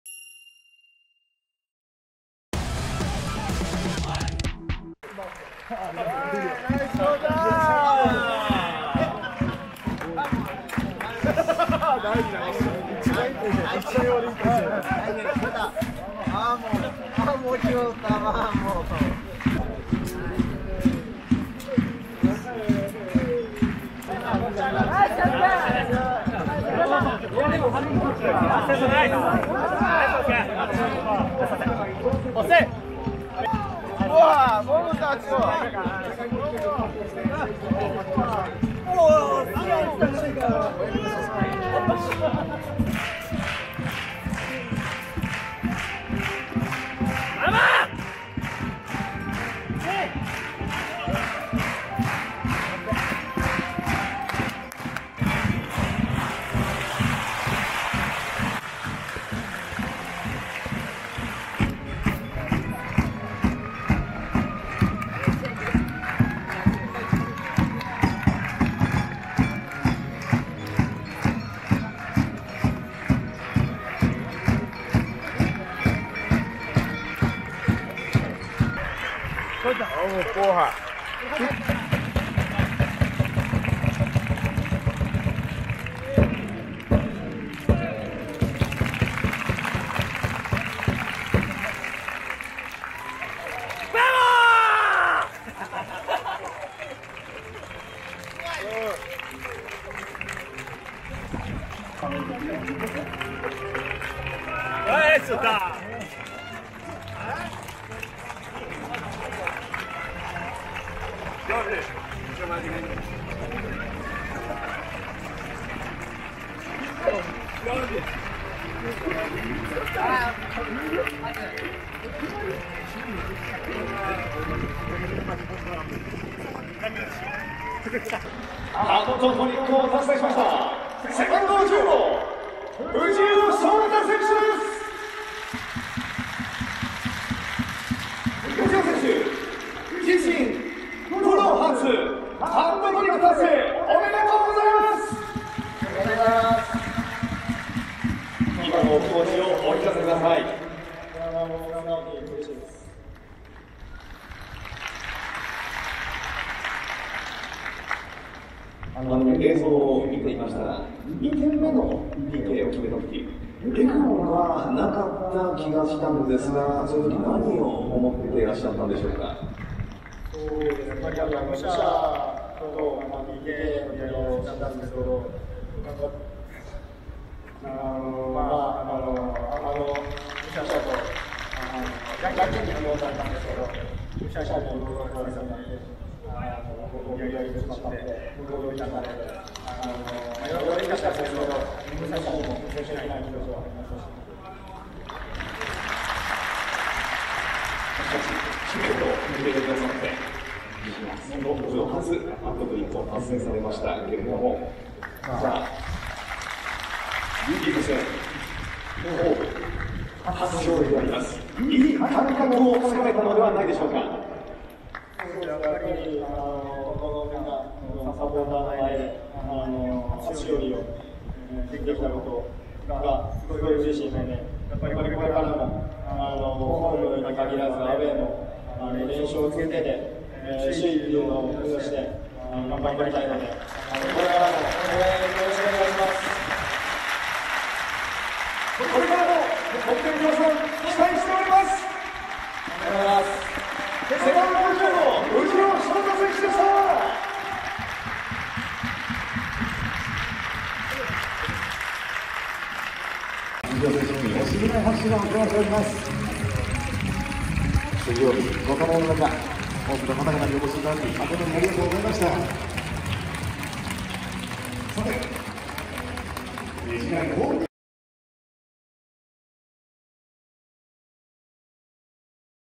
【4/20（水）岩手戦】THE MOMENT
【4/20（水）vs いわてグルージャ盛岡戦】
勝利が決まった瞬間の選手たちの様子をお届けします。